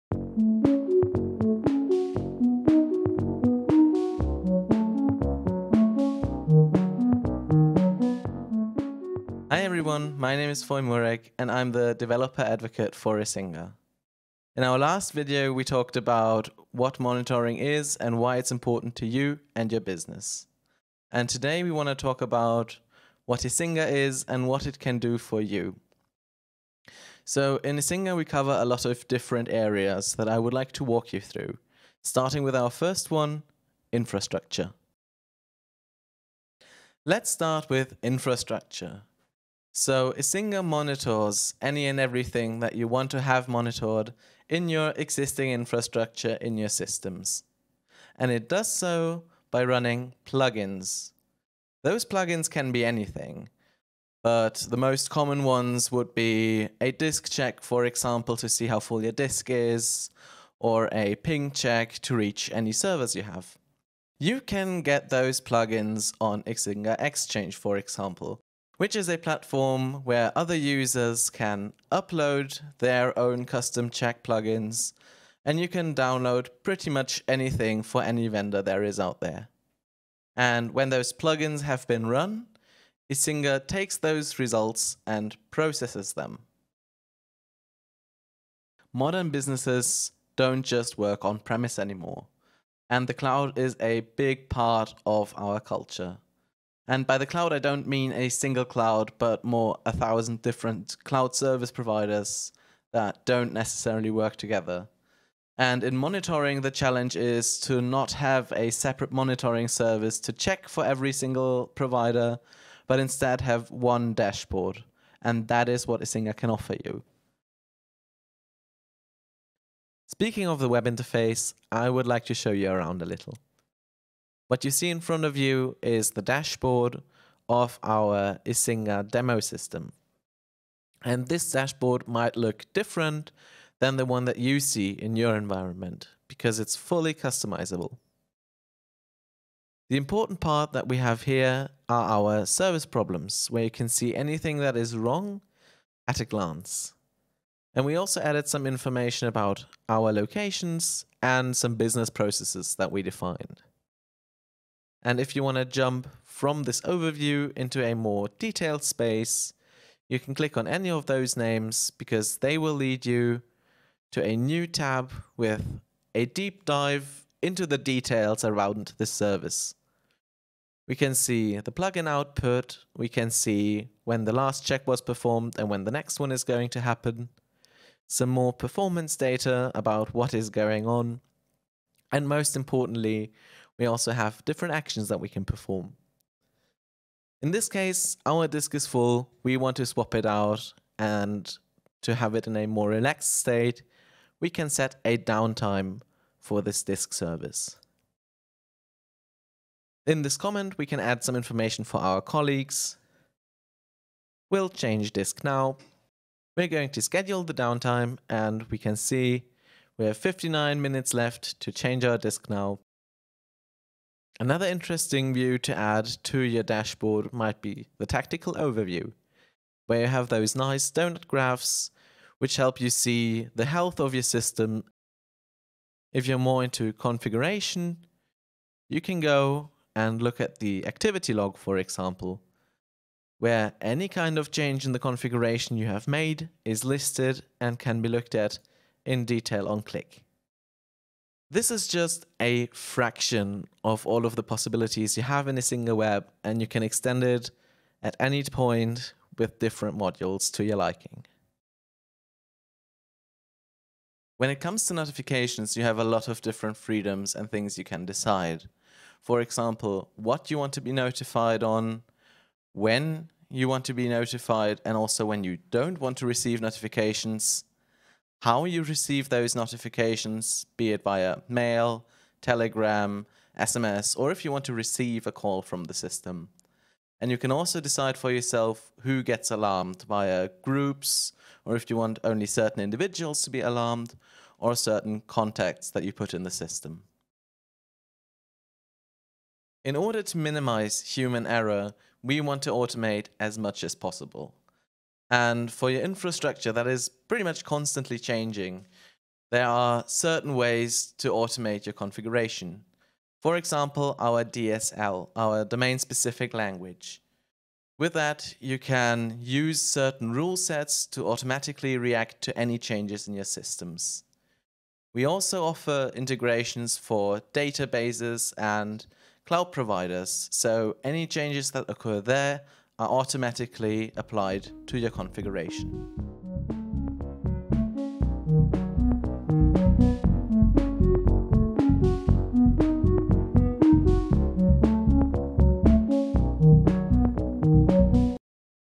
Why would you want to use Icinga?
We tackled the question "Why is montioring important?" before, now it is time to take a look at Icinga.
Learn more about what we can do for you in this video or on our